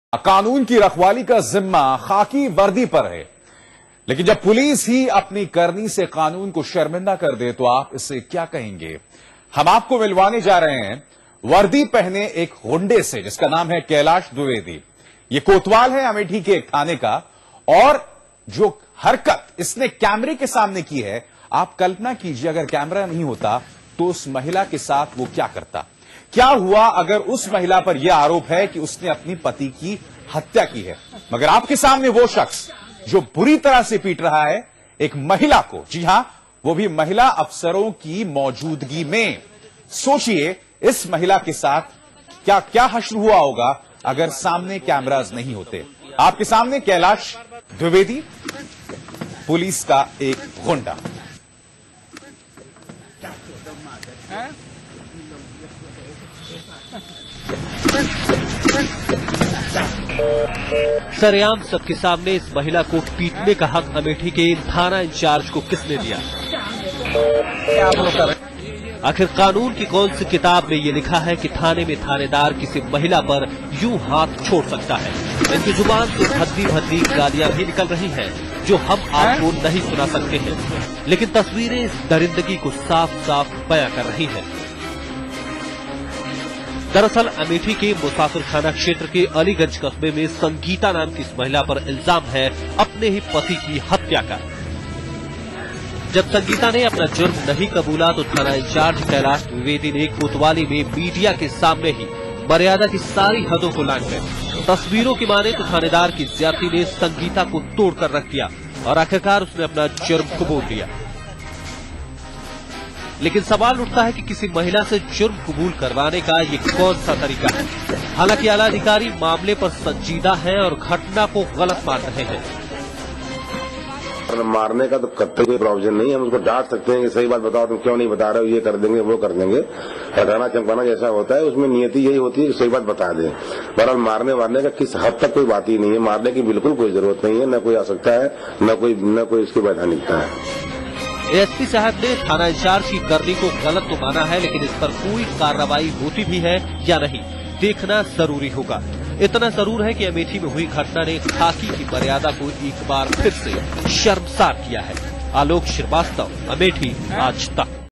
Amethi
Amethi in the case throws a police station where a woman in charge of the station starting to bill thrown Tabrtod slap. Nor in terms of dignity is not legal. While the news media after coming to Saco has been Spend